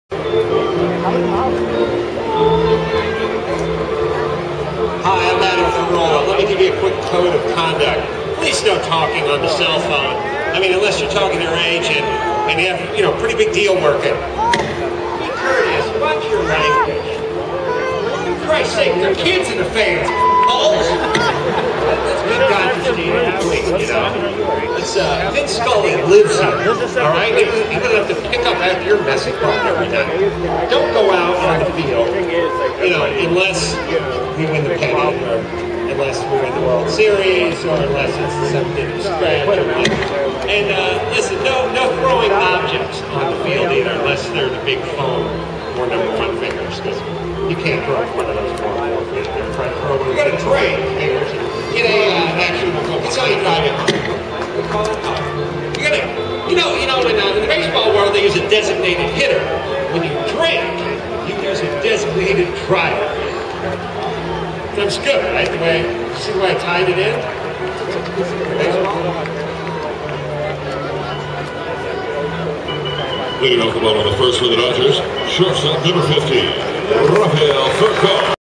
Nancy Bea Time at Dodger Stadium 9-18-09
Some nancy bea organ on 9/18/09  sorry the guy on the diamond vision voice on the background